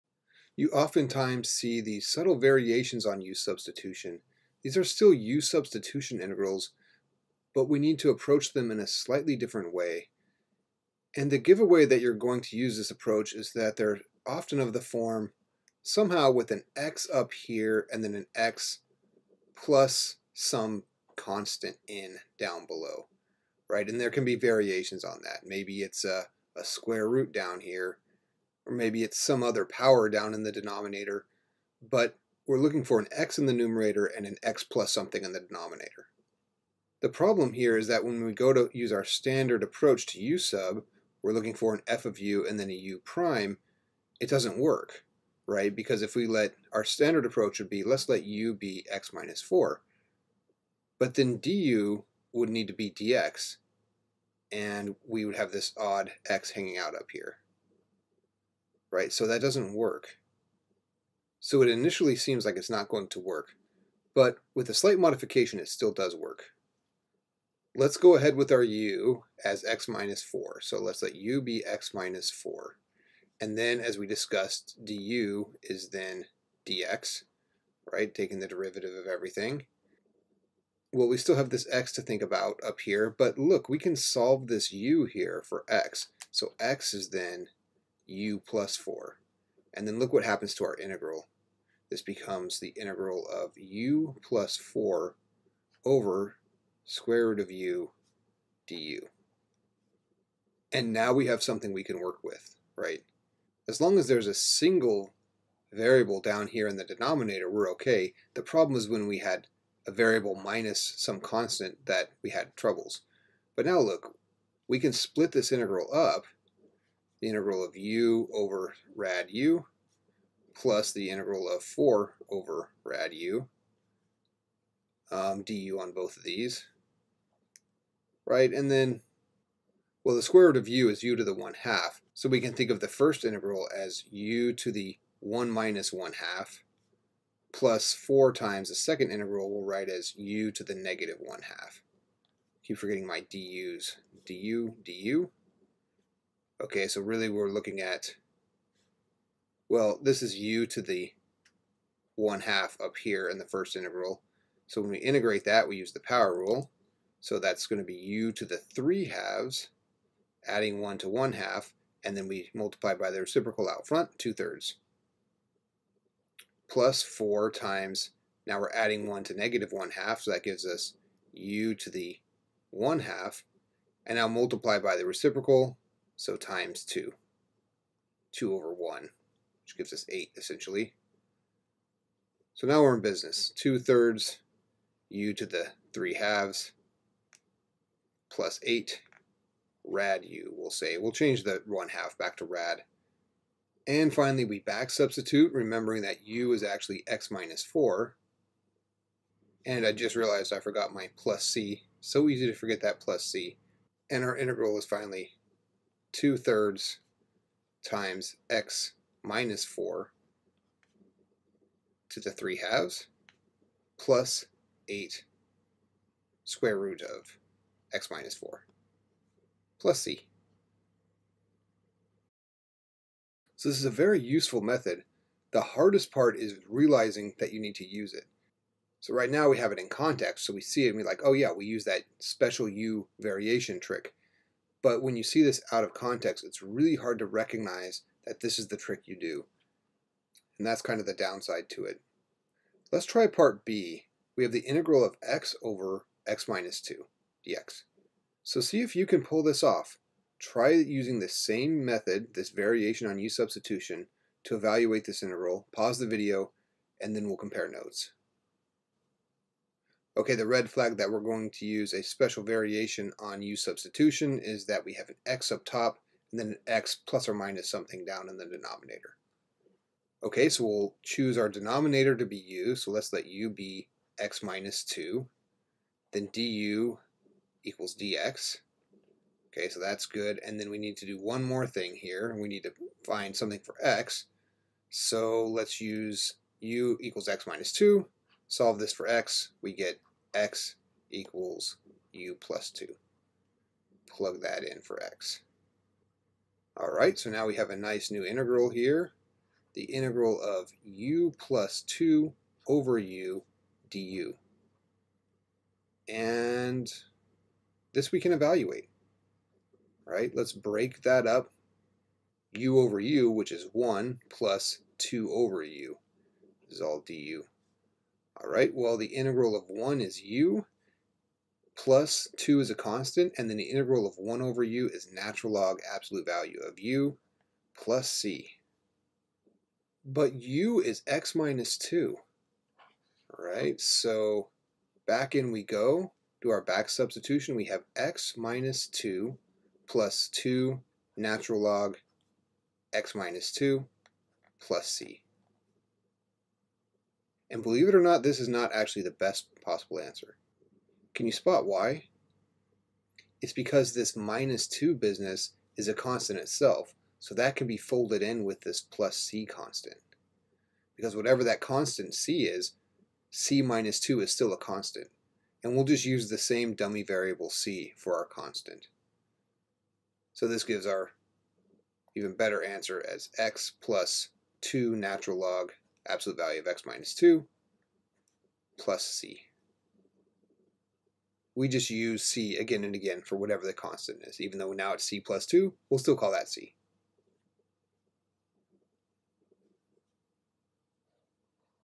Challenging Variations on u-Substitution
Sometimes it’s not clear how to choose u in our u-substitution integrals. This video provides two examples of how to use u-substitution in such challenging cases, such as the integral of a fraction with x in both the numerator and denominator but no x^2. This is a quite useful method, and the hardest part is recognizing that you need to use the nonstandard u-substitution.